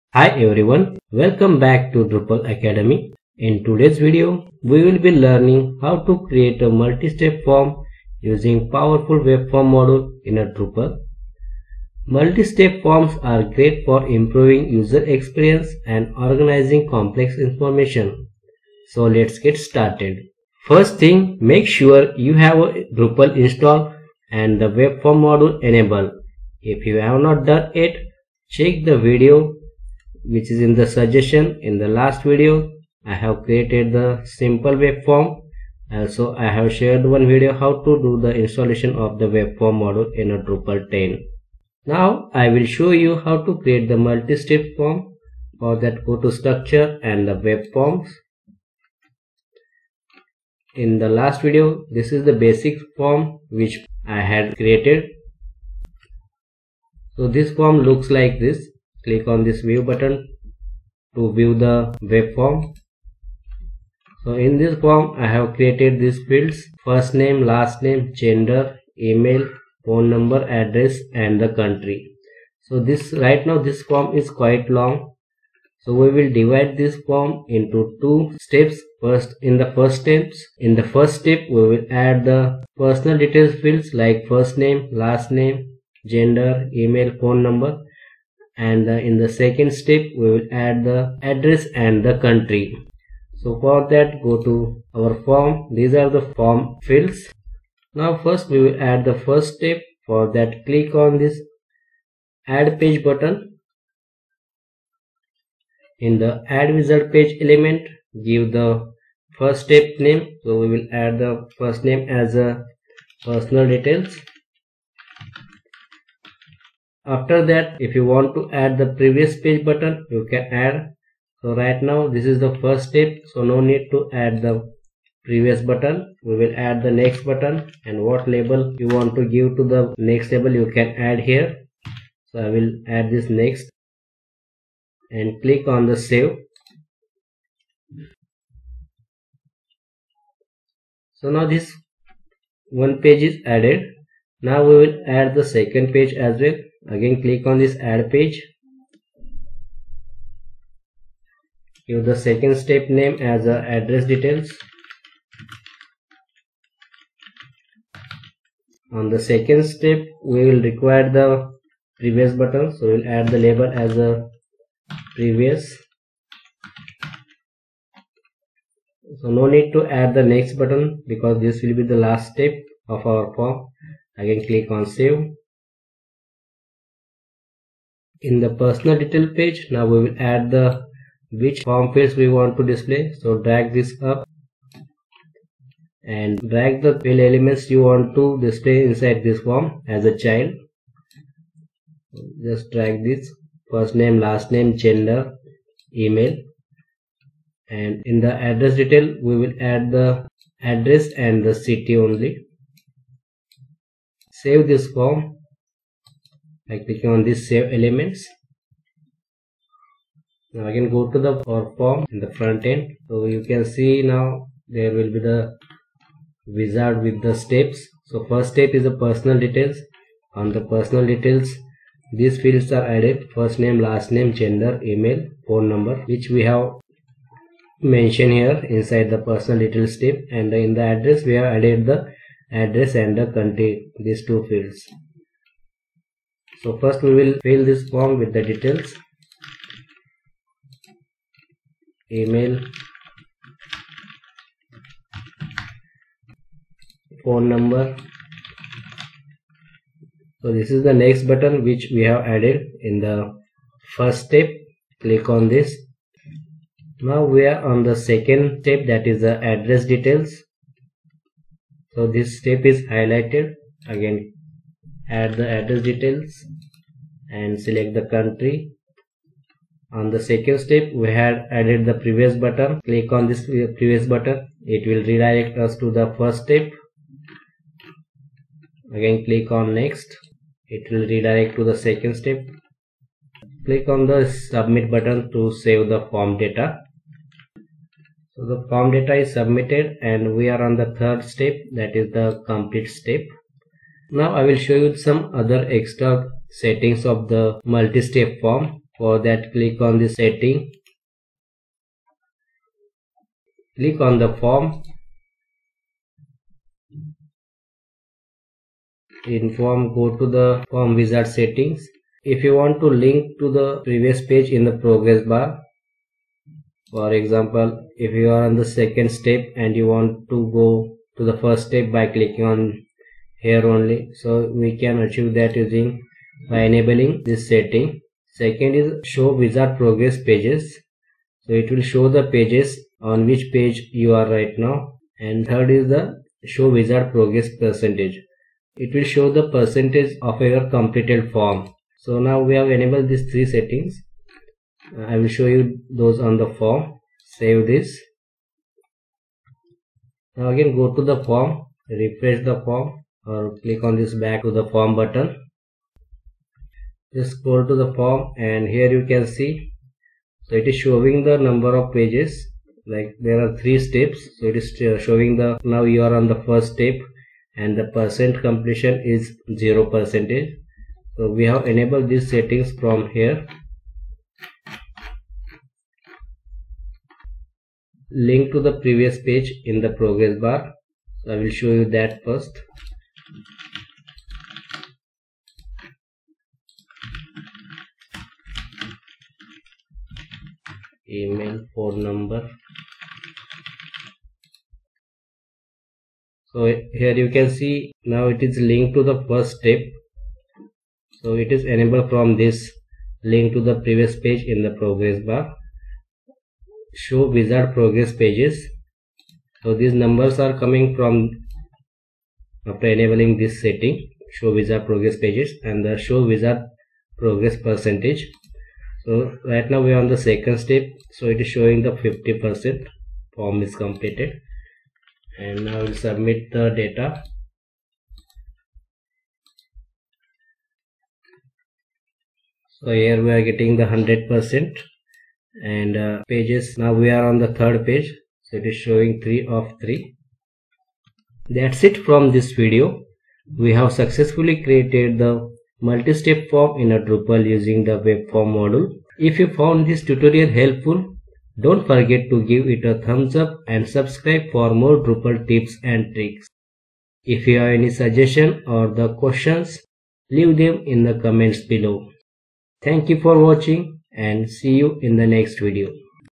Creating Multi Step Form in Drupal Using Webform | Drupal 10 Tutorial
🚀 Creating Multistep Form in Drupal with Webform Tutorial 🚀

Welcome back to Drupal Academy! In this tutorial, we'll guide you through the process of creating a multistep form in Drupal using the powerful Webform module. Multistep forms are fantastic for improving user experience and organizing complex information, and we'll show you the step-by-step process to set it up.

📝 Summary:

Learn how to create a basic webform in Drupal.
Discover how to turn a basic form into a multistep form using the Webform module.
Organize your form into pages and configure page navigation.
Enhance your form for a personalized user experience.

💬 Have questions or suggestions? Leave them in the comments below, and we'll get back to you!